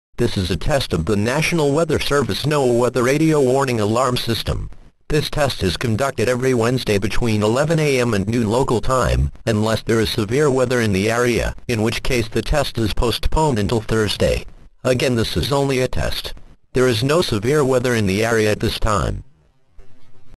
TOM_RWT_CRP.m4a
Tom reading the Required Weekly Test Message from the National Weather Service Office in Corpus Christi. VoiceText Paul will read it tomorrow (08/11/2020) on WXK26 at 11 AM CDT.

This is a test of the National Weather Service NOAA Weather Radio Warning Alarm System. This test is conducted every Wednesday between 11 AM and noon local time, unless there is severe weather in the area, in which case the test is postponed until Thursday. Again this is only a test. There is no severe weather in the area at this time.